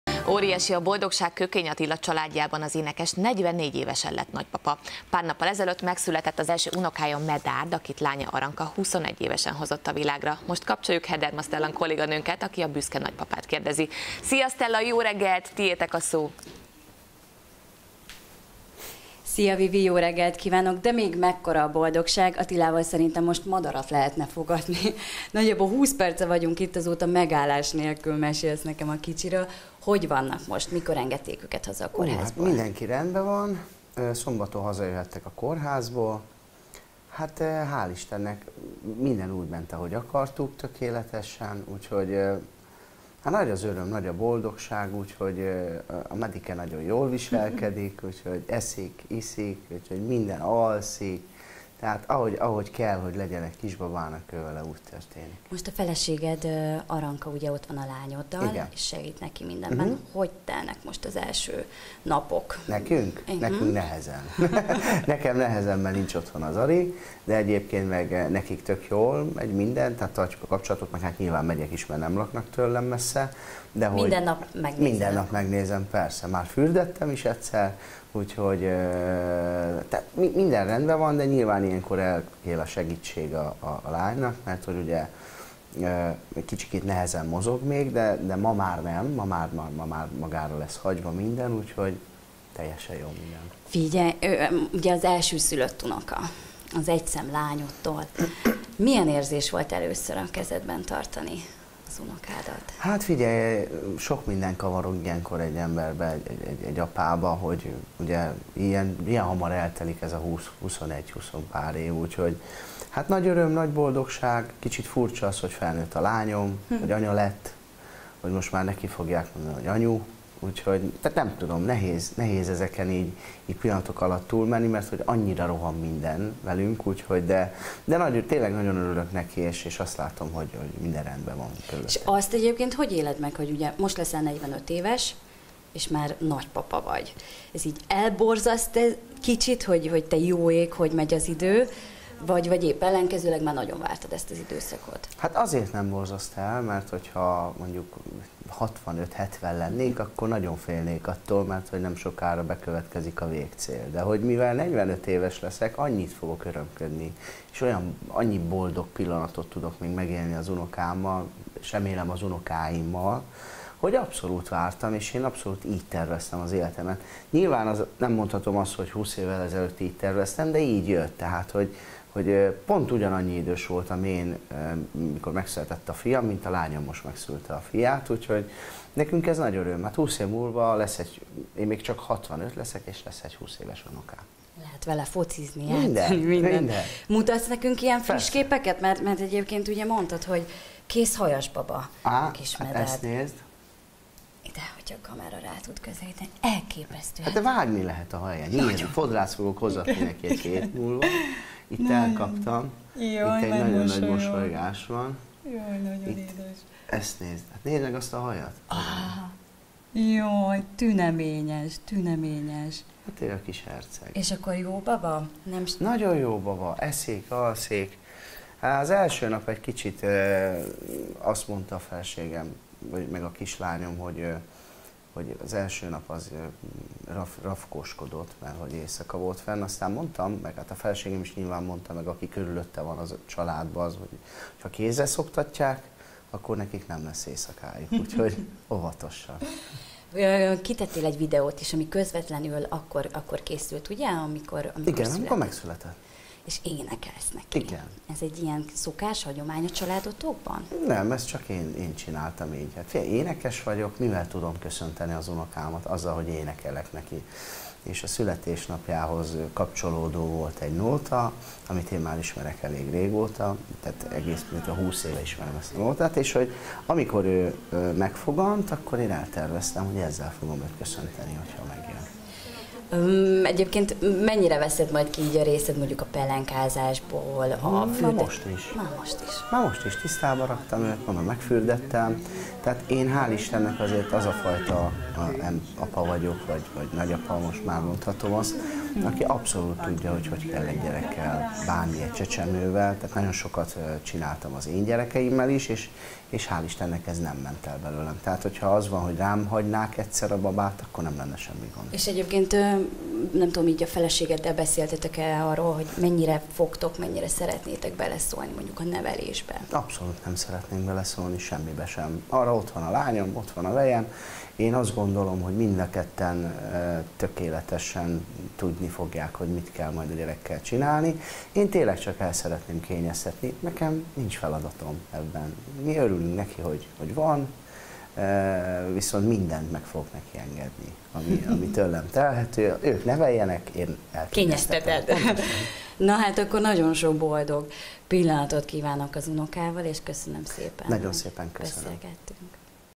Intim családi felvételt osztott meg unokája születéséről a 44 éves Kökény Attila
Madarat lehetne fogatni Kökény Attilával, akinek újszülött unokáját szombaton engedték haza a kórházból. Az énekes elmondta, hogy bizony ez számukra is egy nehéz időszak, ugyanis rengeteg teendőjük van a pici körül, mégis óriási örömmel és boldogsággal gondozzák a picit.